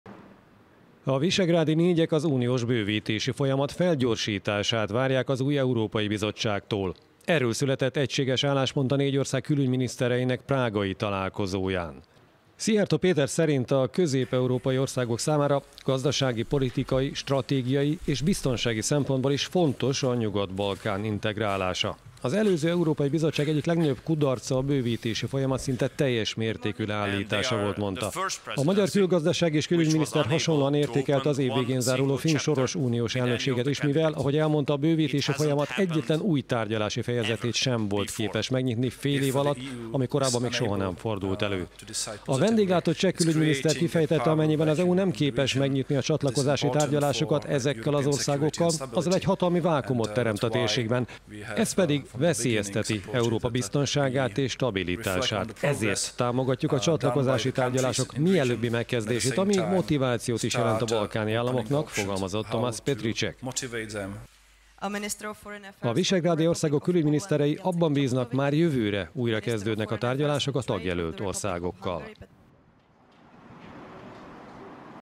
A V4-ek az EU bővítését szorgalmazzák
Egységes álláspont a külügyminiszteri találkozón.…